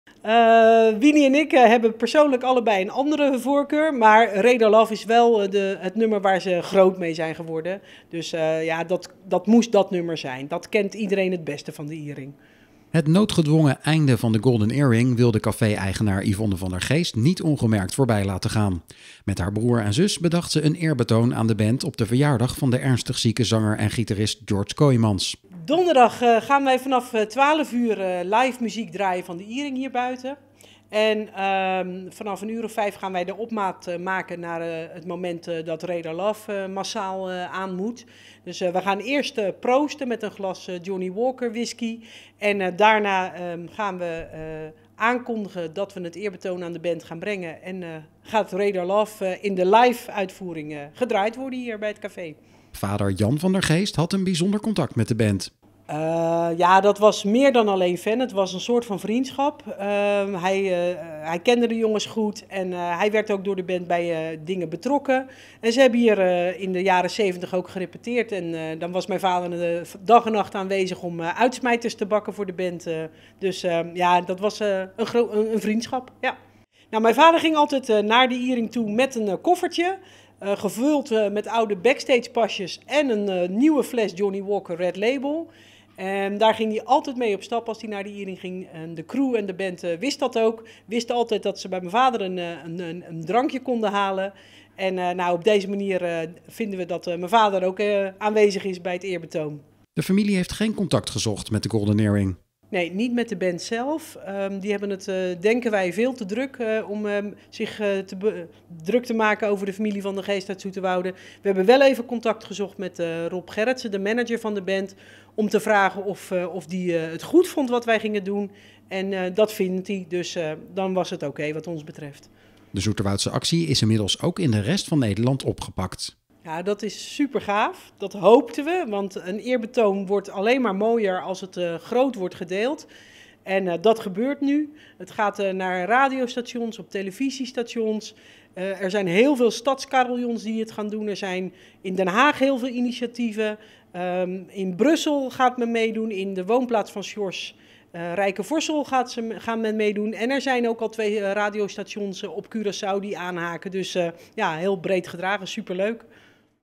Caféhouder Yvonne van der Geest over eerbetoon Golden Earring (2021-03-08)
Caféhouder Yvonne van der Geest uit Zoeterwoude organiseerde een eerbetoon aan George Kooymans en de Golden Earring.